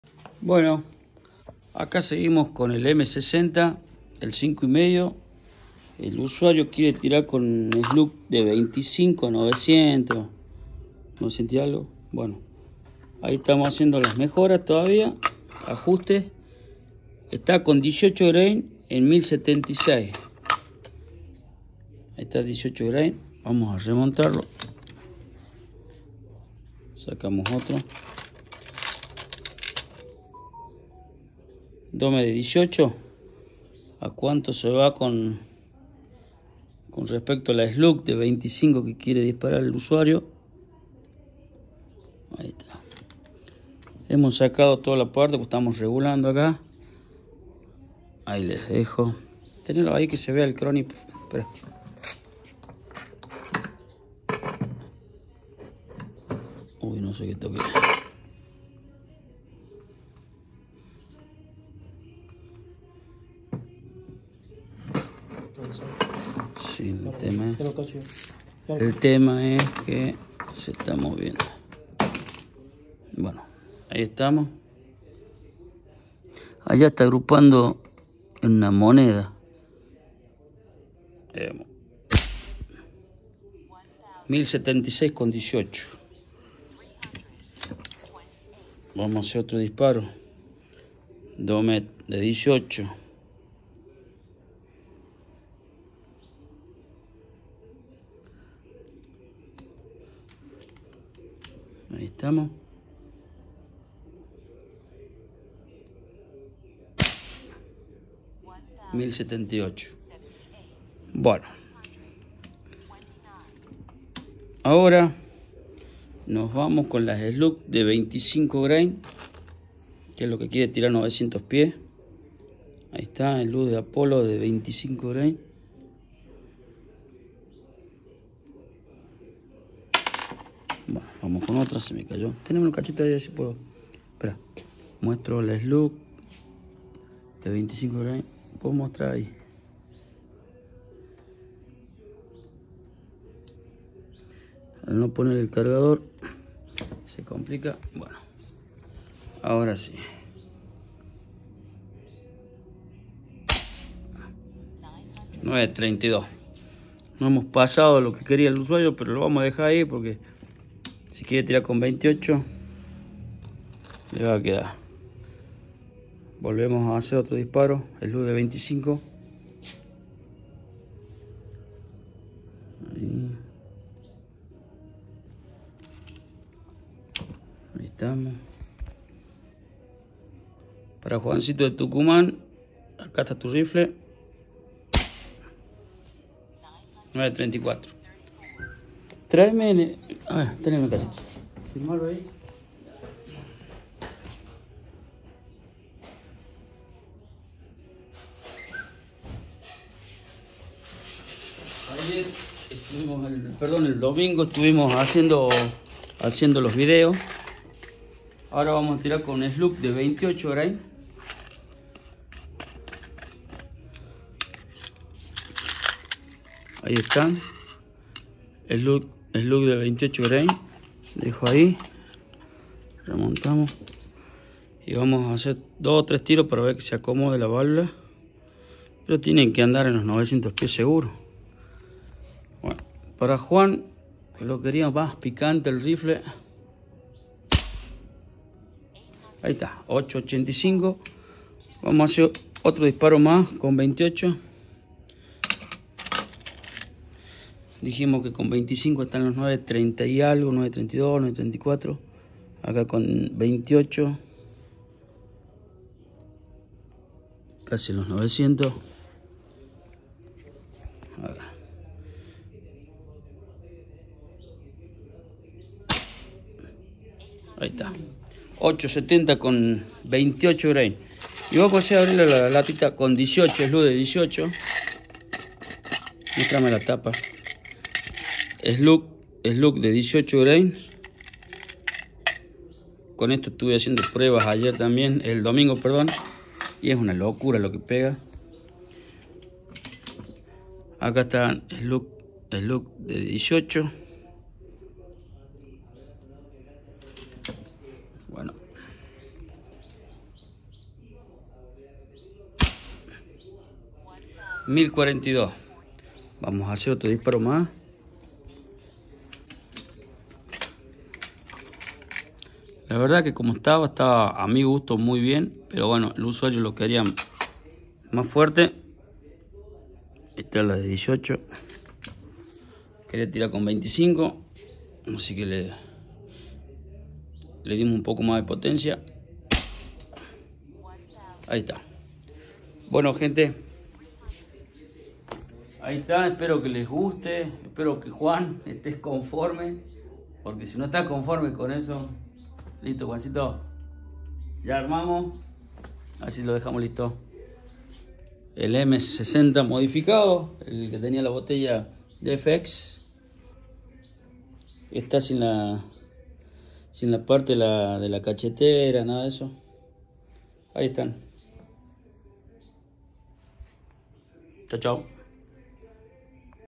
Más velocidad Para el M60 a pedido del usuario ,1078 FPS con Domed de 18grs - 934 FPS Slug 25 grs
# Artemis M60 # snowpeak M60 # Fox M60 # más velocidad !!